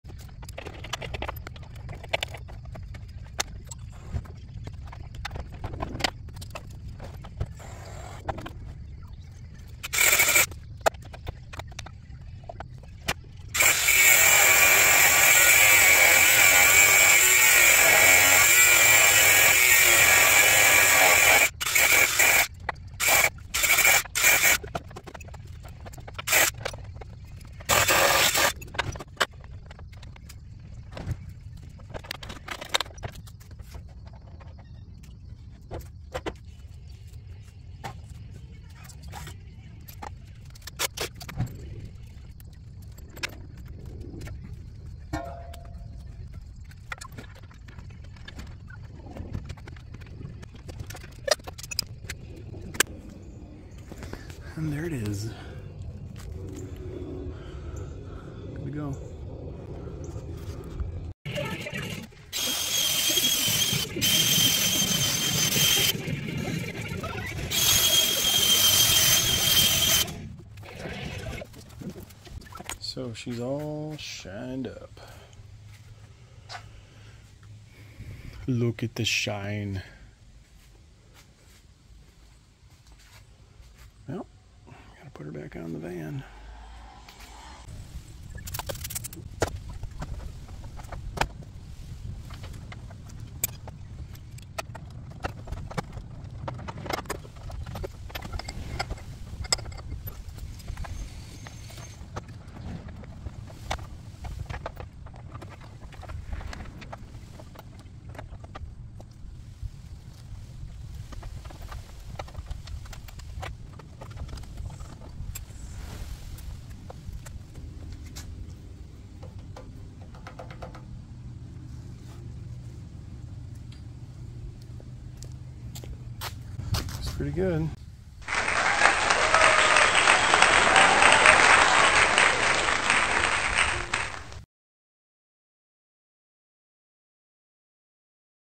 installing a Curt front hitch on a camper van. Easy peazy!
if you want to carry bikes, and open your rear doors, put on a front hitch.   it only takes a few hours, a torch, and a grinder!

Watch as I install a Curtis hitch on my Ford 1999 Okanagan camper van!